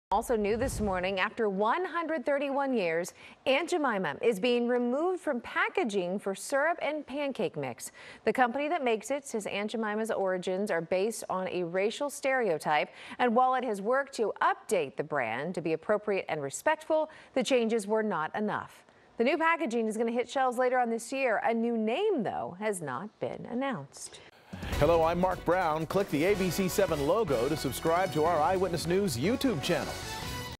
Aunt Jemima: Quaker Oats to change Aunt Jemima brand name, images due to racial stereotypes | ABC7
Pepsico is changing the name and images associated with its Aunt Jemima brand, acknowledging its racist origins. The parent company of Quaker Oats announced Friday that the brand will get new branding and new packaging before the end of 2020.